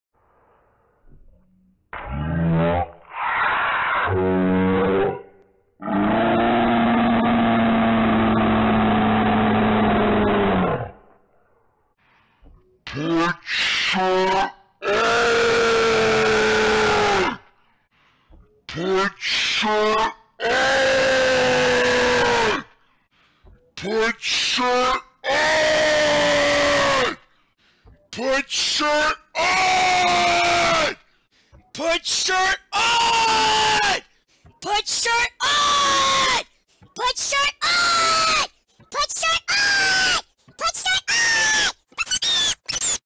Bill Jensen Yells "Put Shirt On" But I Mess With His Voice
Something stupid I made for laughs lmao..since people love to hear Bill rage ^w^